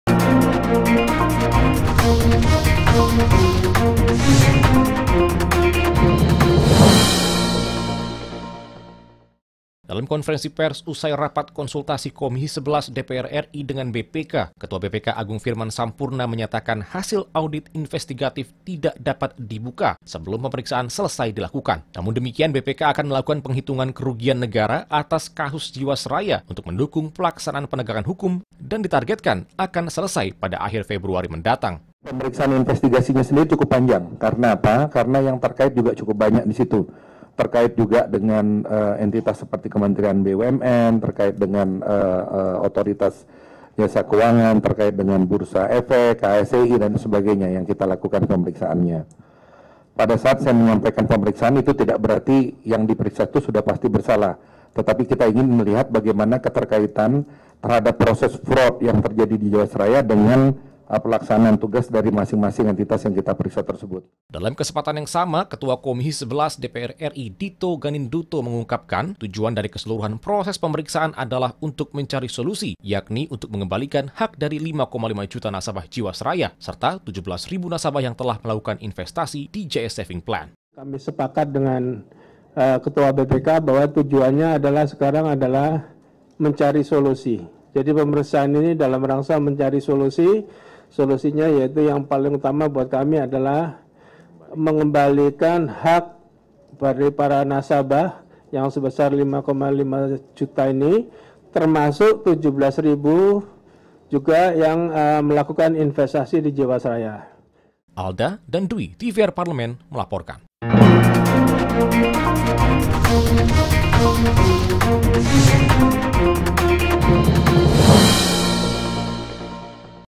WARTA PARLEMEN - DPR RI - PENGHITUNGAN KERUGIAN NEGARA SELESAI AKHIR FEBRUARI
// BADAN PEMERIKSA KEUANGAN AKAN MENGHITUNG SECARA KOMPREHENSIF KERUGIAN NEGARA YANG TIMBUL AKIBAT KASUS JIWASRAYA// SEMENTARA ITU/ KOMISI XI DPR RI TERUS MENCARI SOLUSI PENYELESAIAN DARI KASUS INI//

NARSUM : AGUNG FIRMAN SAMPURNA / Ketua BPK RI
DITO GANINDUTO / Ketua Komisi XI DPR RI / F-PG / Dapil JATENG VIII